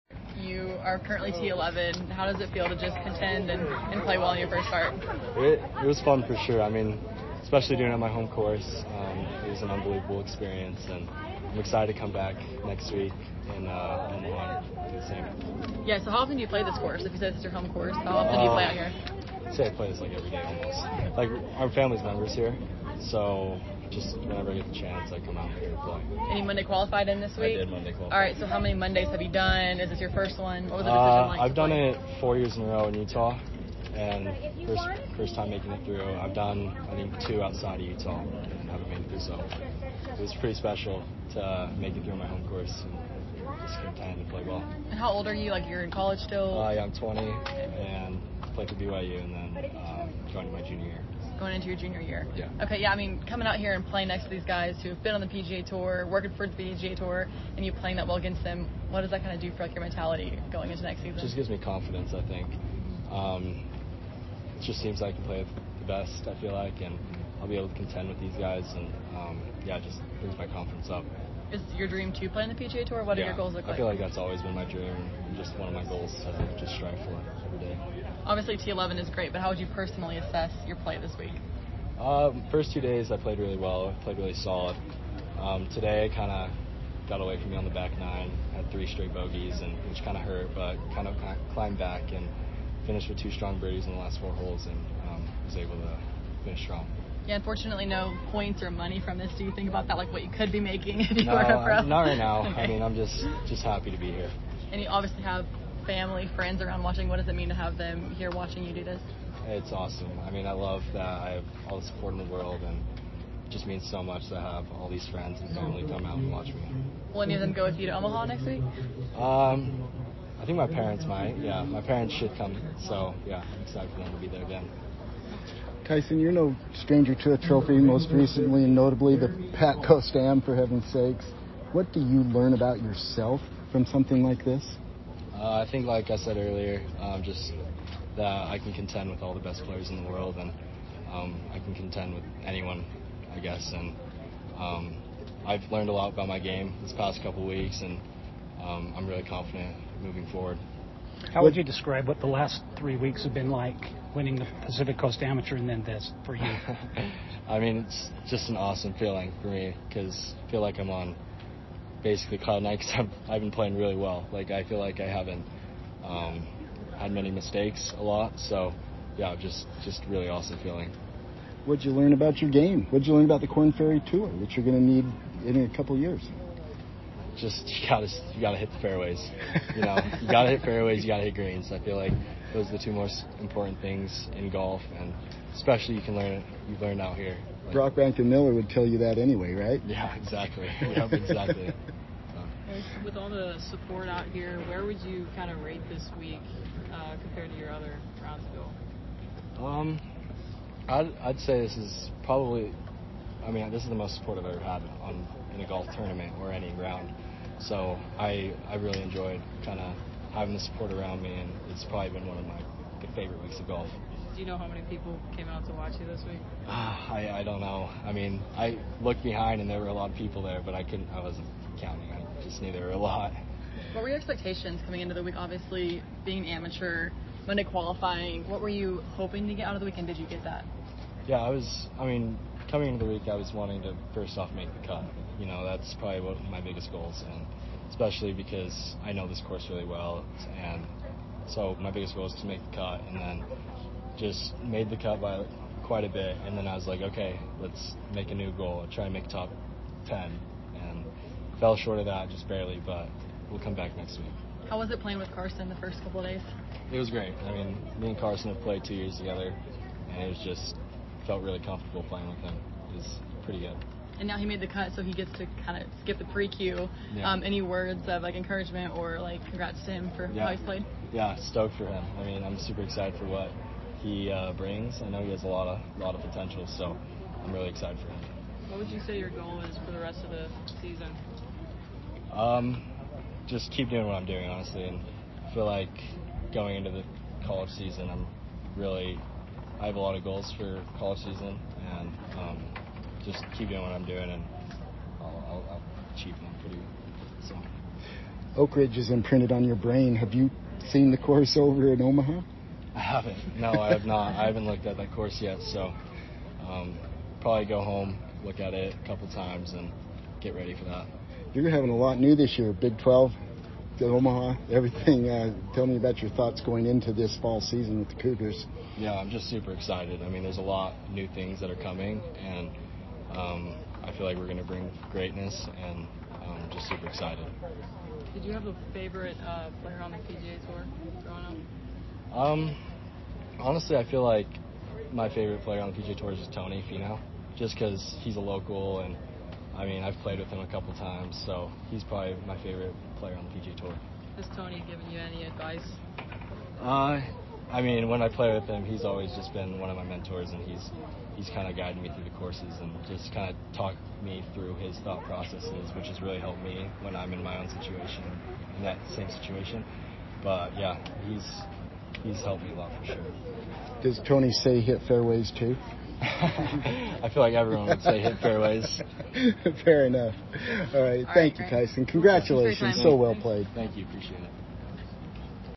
BYU golfer Tyson Shelley shoots 17-under in first Korn Ferry Tour start
BYU golfer Tyson Shelley speaks with the media after firing a four-day total of 17-under-par 267 to finish in the top 25 at the Utah Championship, the first start on the Korn Ferry Tour for the rising junior that clinched a spot in the field at next week's Pinnacle Bank Championship in Omaha, Nebraska.